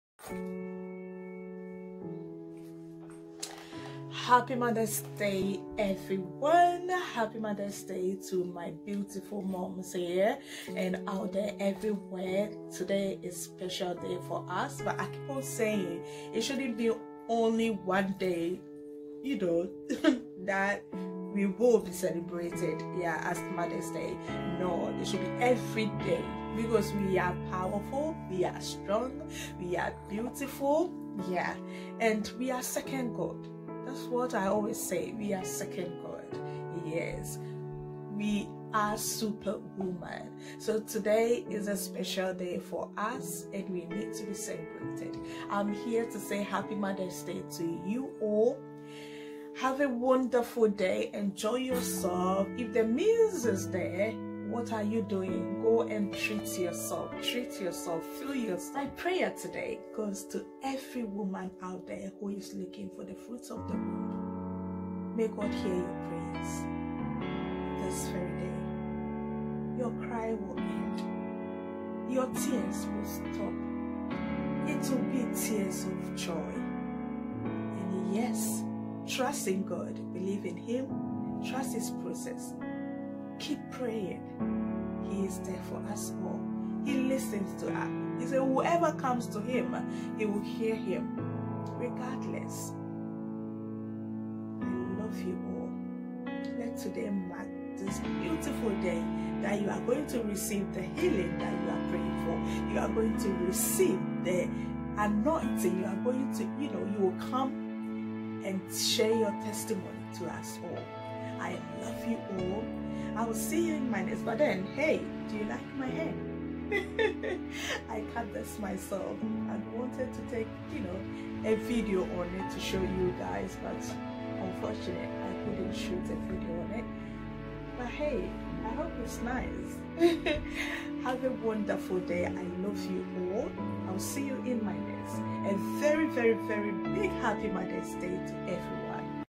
FROM ME TO YOU HAPPY MOTHERS DAY BEAUTIFUL MOMS 💖
To my beautiful mums here, everywhere  today is a special day for us all to be celebrated. May this day mark a special day you will forever cherish.

I pray every woman here everywhere who is praying for the fruit of the womb be answered in Jesus Mighty Name Amen 🙏🏽 HAPPY MOTHERS DAY ❤

God bless us all as we celebrate this day 🙏🏽😊😘